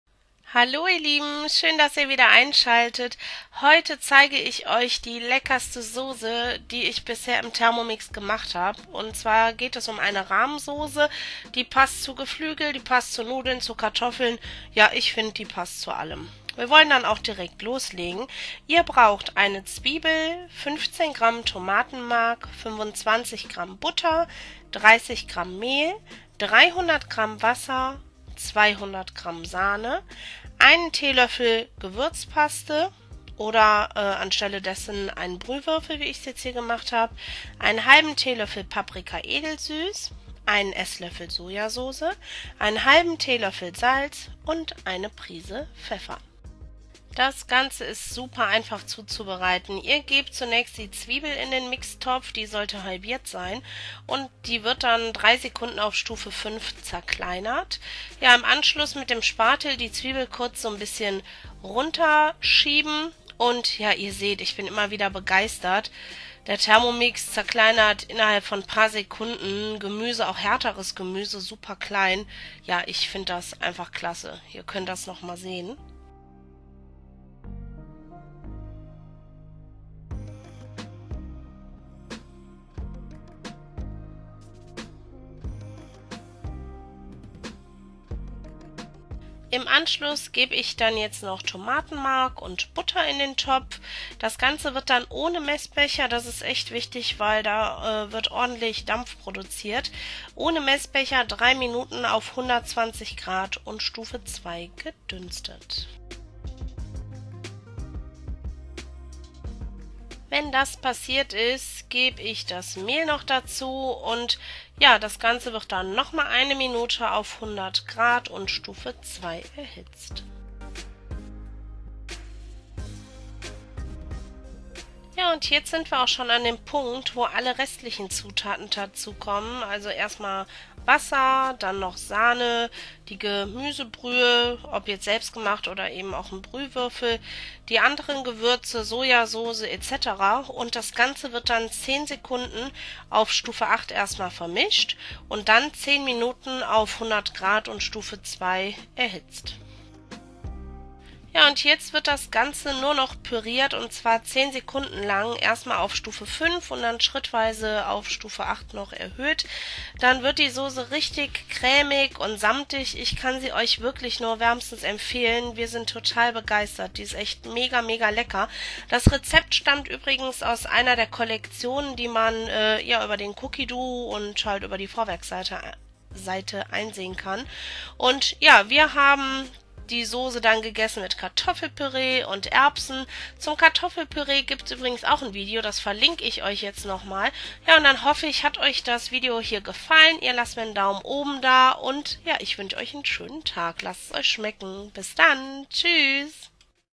* Rahmsoße * Thermomix * Mega lecker *
heute zeige ich euch eine schnelle hausgemachte Rahmsoße aus dem Thermomix. Die  Soße passt zu Geflügel, Kartoffeln, Pasta, einfach allem :) Sie ist echt mega lecker, probiert´s mal aus. Das Rezept stammt übrigens aus einer der Thermomix Kollektionen.

Equipment:
Kamera: Sony DSC-HX90
Stativ: Hama